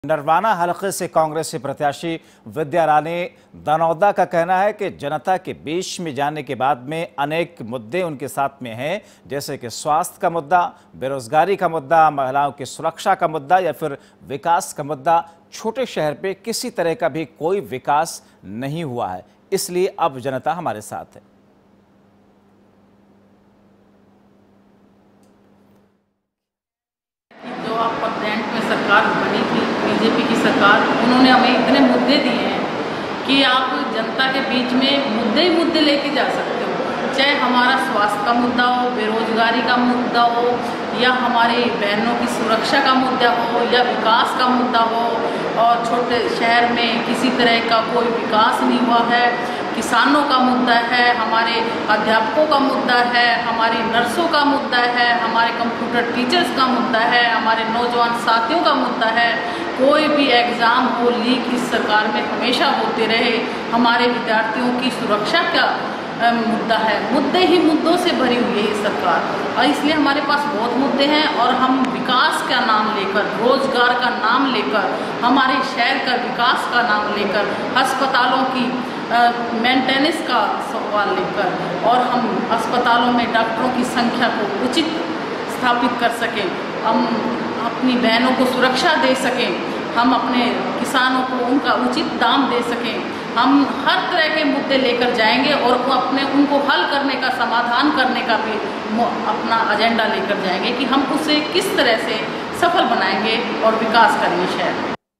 NARWANA: कांग्रेस प्रत्याशी विधारानी दनौदा का बयान- 'हलके का नही हुआ विकास'
नरवाना हलके से कांग्रेसी प्रत्याशी विद्यारानी दनौदा का कहना है कि जनता के बीच में जाने के बाद अनेक मुद्दे हैं...स्वास्थ्य का मुद्दा, बेरोजगारी का मुद्दा, महिलाओं की सुरक्षा का मुद्दा हो या विकास का मुद्दा छोटे शहर में किसी तरह का भी कोई विकास नहीं हुआ है.